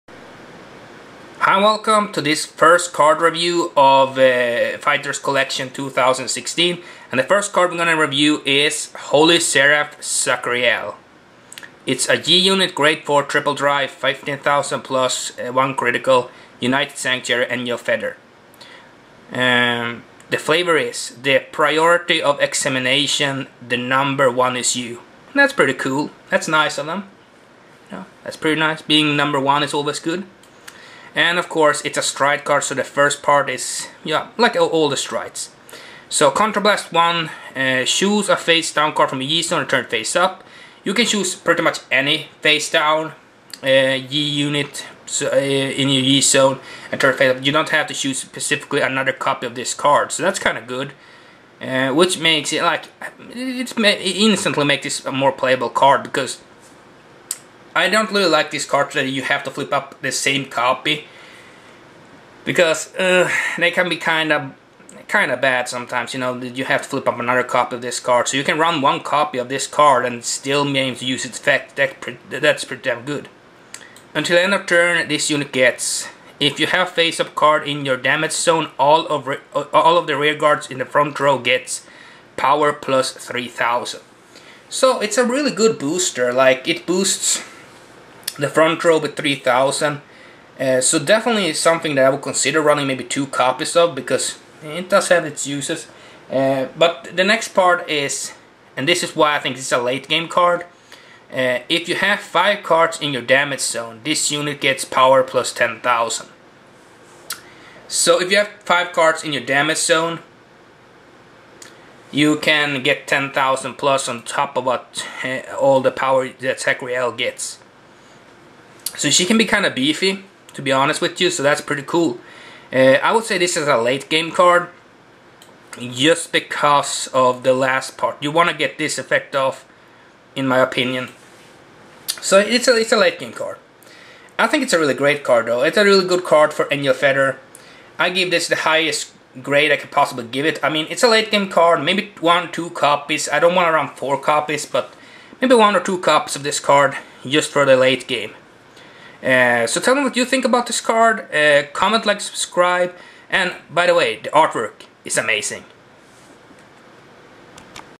Stand Up Cardfight Vanguard Card Review: Holy Seraph Zachariel
Holy Seraph, Zachariel
Flavor: The priority of extermination. the number one is you.
G Unit
Grade 4 / Triple Drive!!!
Attack: 15000+
★1
United Sanctuary
Angel Feather

[Stride]-Stride Step-[Choose one or more cards with the sum of their grades being 3 or greater from your hand, and discard them] Stride this card on your (VC) from face down.
[ACT](VC)[1/Turn]:[Counter Blast (1) & Choose a face down card from your G zone, and turn it face up] Until end of turn, this unit gets "[CONT](VC):If you have a face up card in your damage zone, all of the rear-guards in your front row get [Power]+3000. If you have five cards in your damage zone, this unit gets [Power]+10000.".